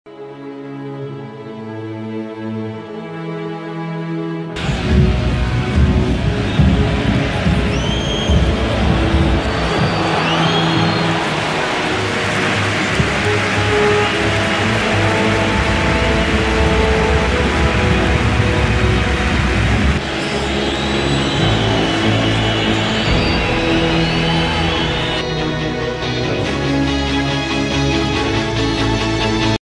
02 ROGER: Los 50 mejores goles del RCDE en Liga
Gol de Roger García. Rayo Vallecano - RCD Espanyol. Temporada 2002/03.